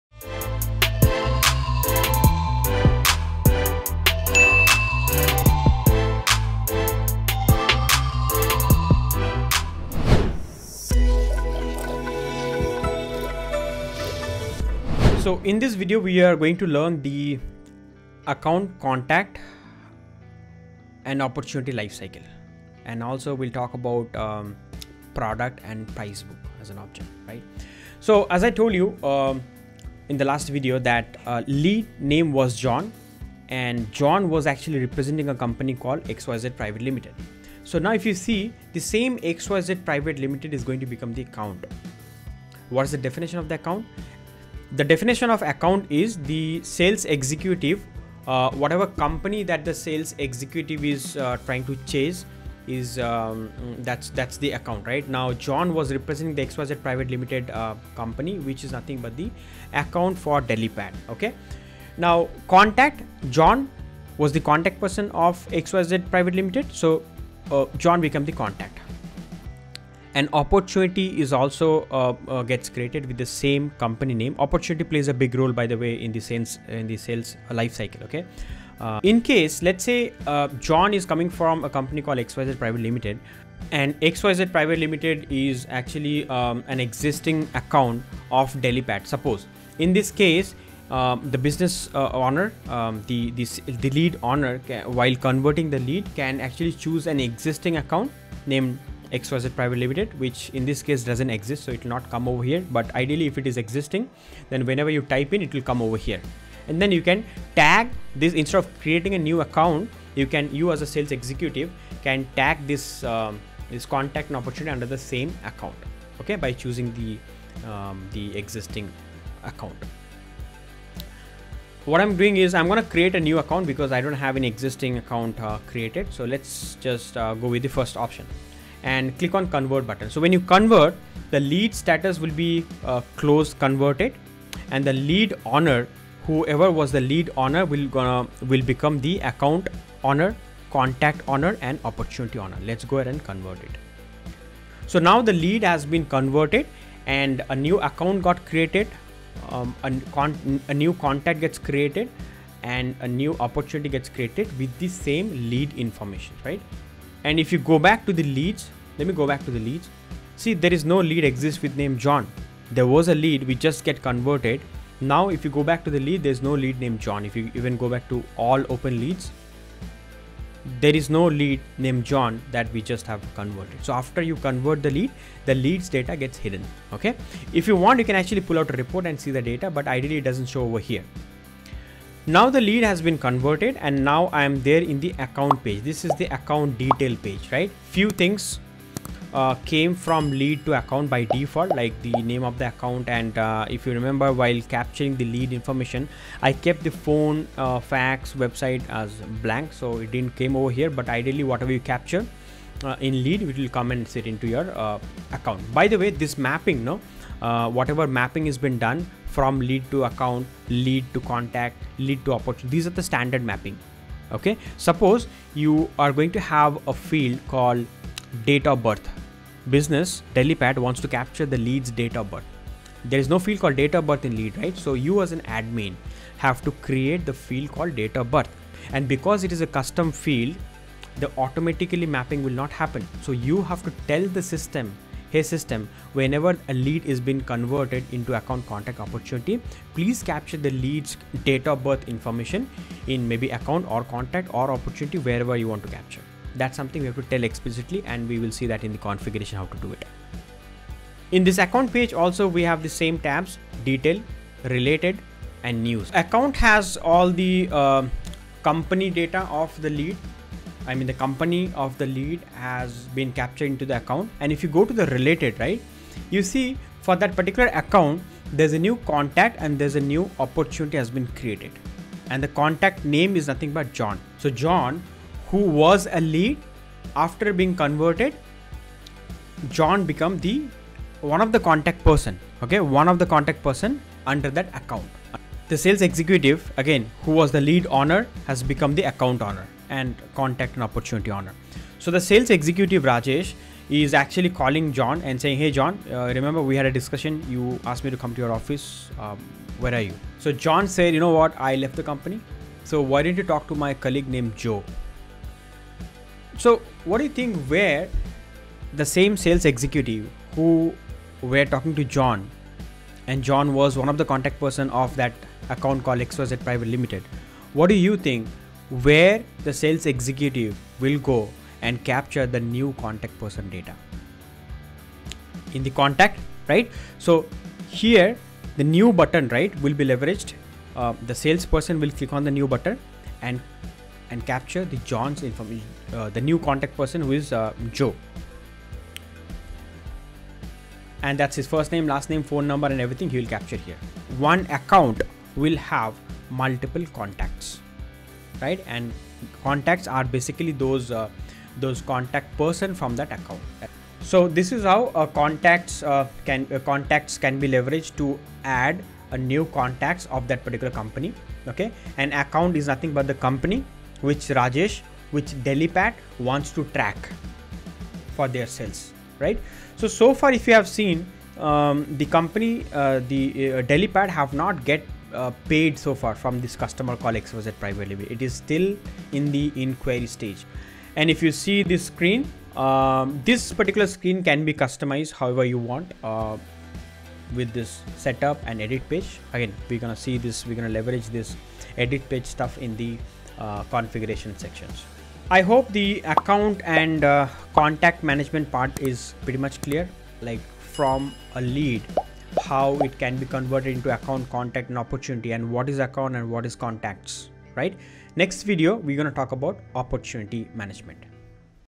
Account and Contact Management in Salesforce || Salesforce Tutorial || Delipat || Rajesh Chatterjee
Lead is a person showing interest in your product or service. Once a lead is generated, the sales executive talks to the lead and tries to convert him. If the lead shows interest in doing business with you, the lead is converted.

In the last article/video, we discussed the lead life cycle and lead conversion. This article/video will you insights on account and contact in Salesforce.

Once any lead is converted in Salesforce, it becomes account, contact and opportunity. The lead owner (sales executive) becomes the account owner, contact owner and opportunity owner. After conversion, a new account, new contact and new opportunity get created with the existing lead information.

For example, John representing XYZ Pvt. Ltd. is a lead who has been converted. Information of John is then divided and mapped into account, contact and opportunity. Rajesh, the owner of the lead (Sales executive), becomes the owner of the account, contact and opportunity.
Account management in Salesforce
Account refers to the company or business that becomes your customer. In the above example, XYZ Pvt. Ltd. is the account. Upon conversion, a new account is automatically created.

If the business or company is already your customer, their account exists. In such cases, you can tag the new contact and opportunity under the existing account.
Contact Management in Salesforce
Contact is the contact person of the company or business. In the above example, John is the contact. One account can have multiple contacts, just like one business having multiple contact persons.

If you observe we there is no monetary transaction happening. The monetary transaction is an opportunity which we will discuss in the next article/video. I hope this article/video gave you an overview of account and contact management in Salesforce.

⏩ 𝐈𝐦𝐩𝐨𝐫𝐭𝐚𝐧𝐭 Videos